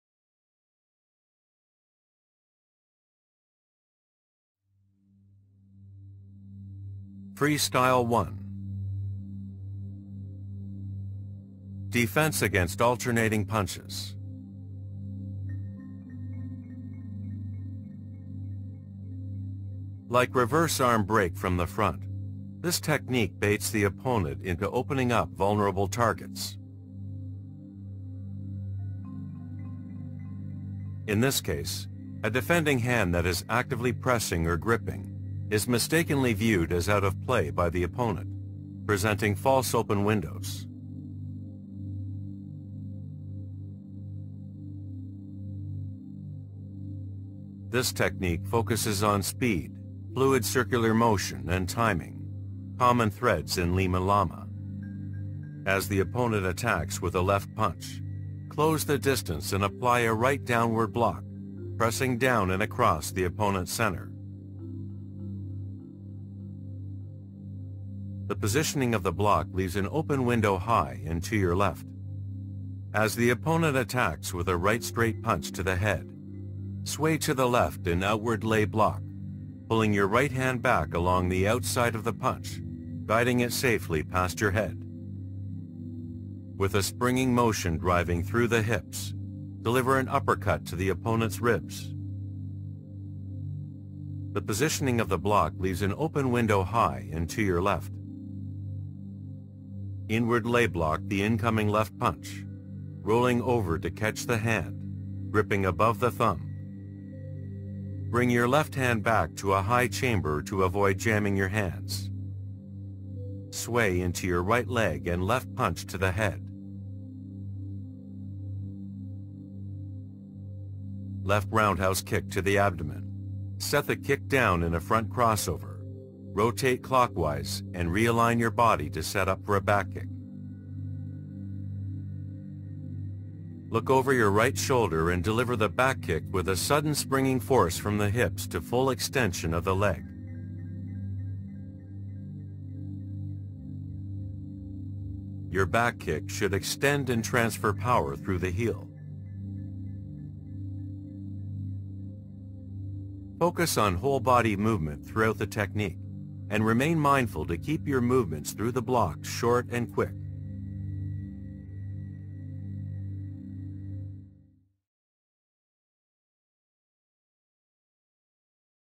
Martial Codex - Freestyle One (Limalama)
Limalama technique "Freestyle One" from the Kata 1 series of the modern TILOA Curriculum presented by SM Rudy Tuiolosega.

The Martial Codex is a 3D Motion Capture Publisher with an application that allows users to follow fully narrated lessons or interact with control over viewing angle and time controls.